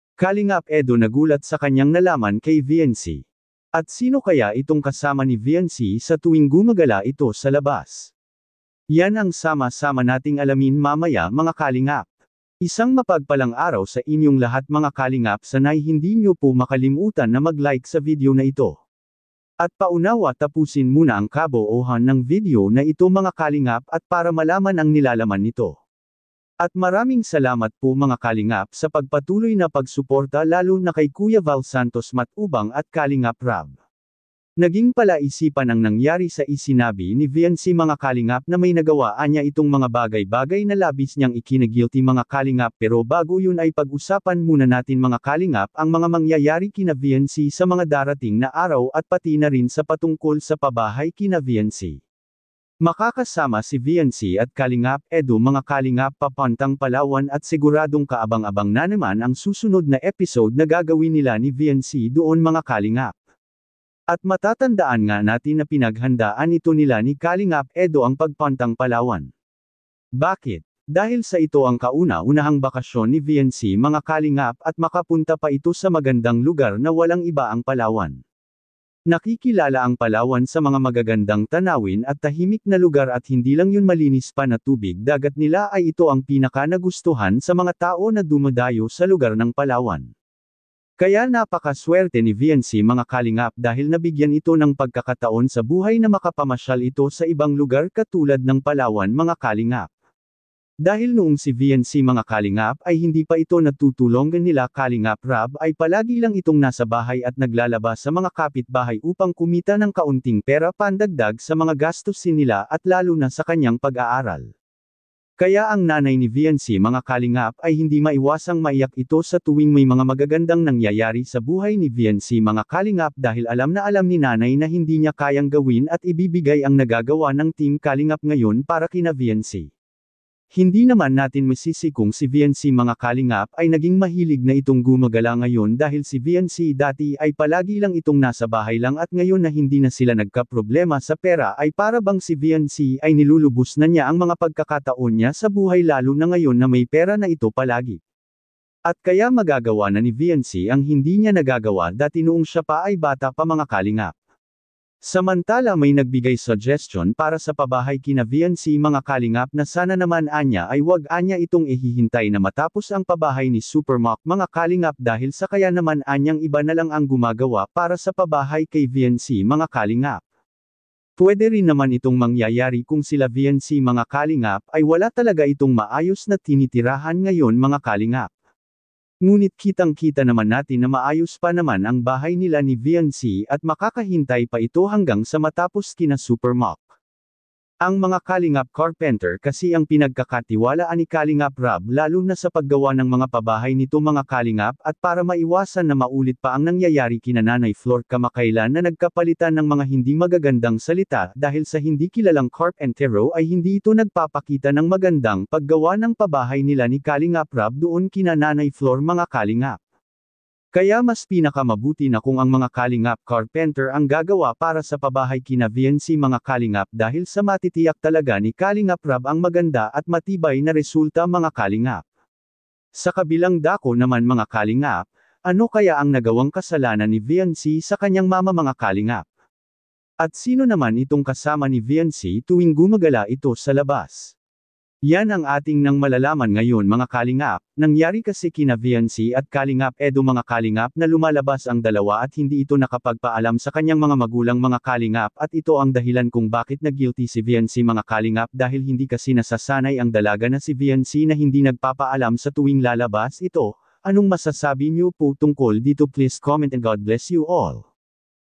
K-EDU NAGULAT SA KANYANG NALAMAN KASAMA NI VEANCY LUMALABAS NALAMAN NA!|KALINGAP RAB LATEST VLOG
nalaman na kung sino ang kasama ni veancy gumagala sa labas!